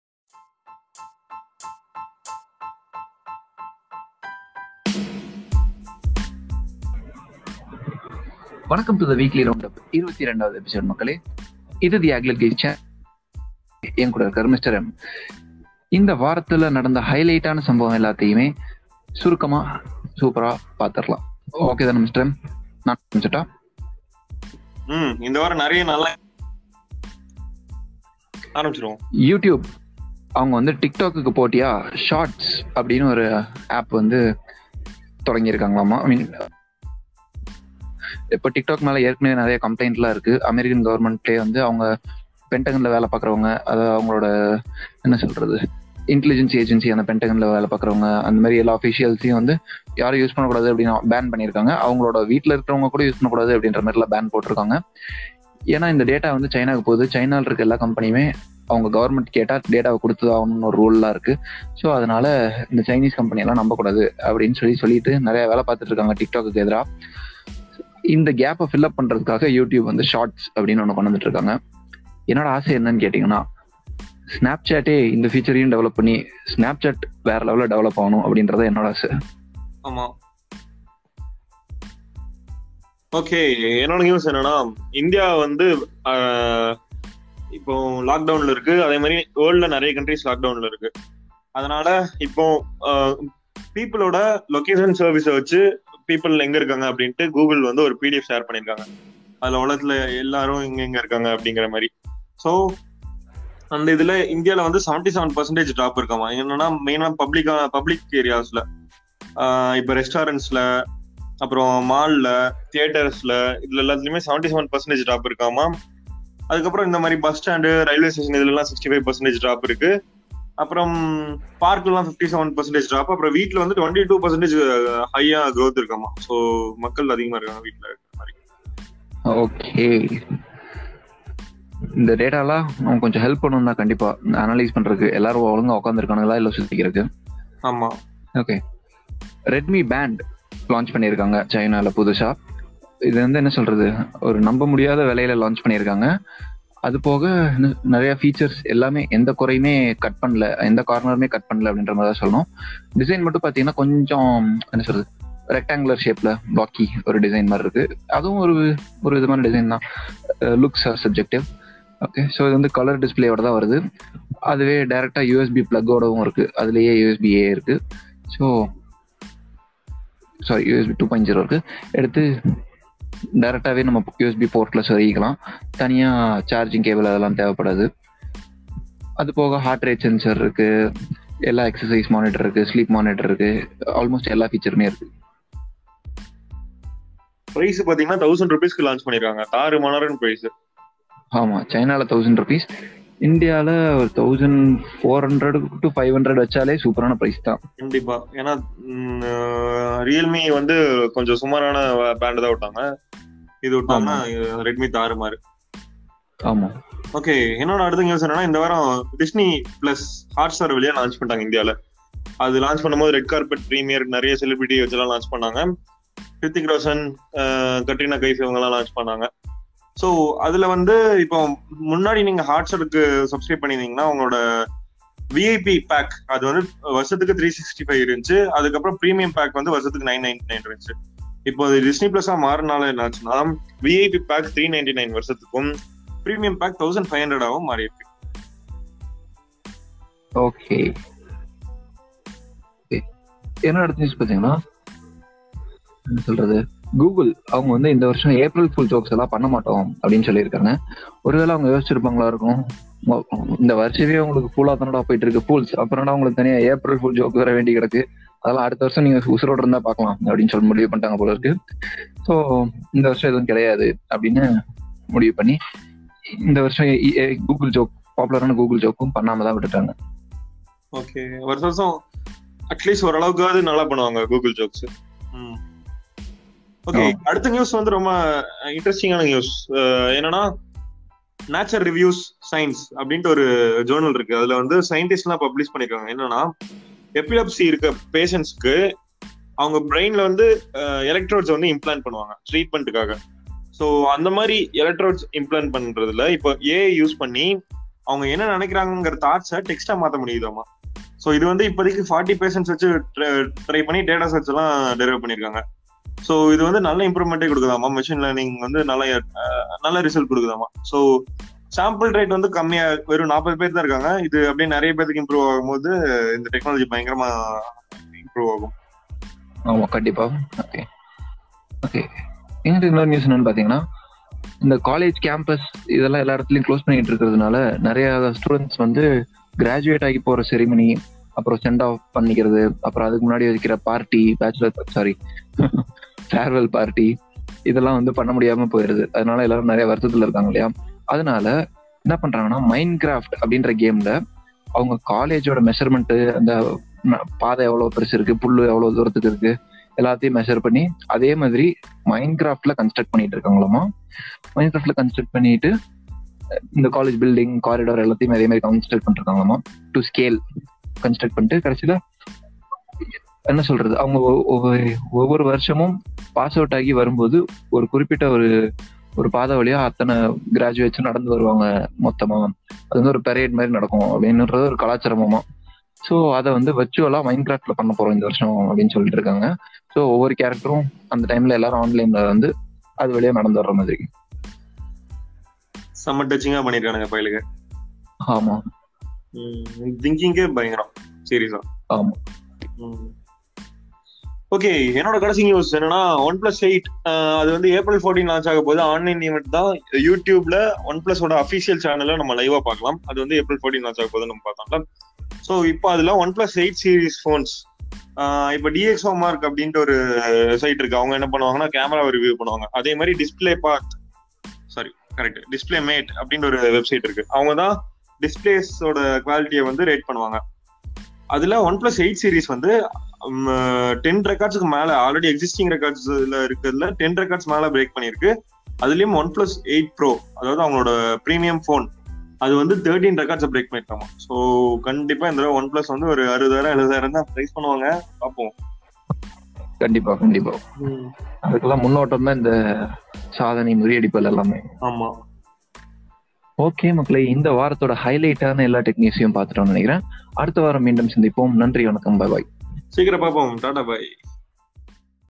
Weekly Tech News #22 OnePlus 8 phones get A+ score from DisplayMate, 8 Pro broke 13 records
2Good news

YouTube is working on “Shorts” to compete with TikTok

Redmi Band With Colour Display, Integrated USB Plug Launched

Disney+ Hotstar Red Carpet Premiere: What and When Is It, and How to Watch

Nature reviews science, Epliepsy patients deployed with electrodes

India Sees a Huge Drop in Visits to Public Places, Google Data Reveals

Google Will Not Make April Fools' Day Jokes This Year Amidst Coronavirus Crisis, Internal Email Suggests

CAMPUS IS CLOSED, SO COLLEGE STUDENTS ARE REBUILDING THEIR SCHOOLS IN MINECRAFT

OnePlus 8 phones get A+ score from DisplayMate, 8 Pro broke 13 records

Redmi Band, YouTube Shorts, India stays at home, No April fool for Google, OnePlus 8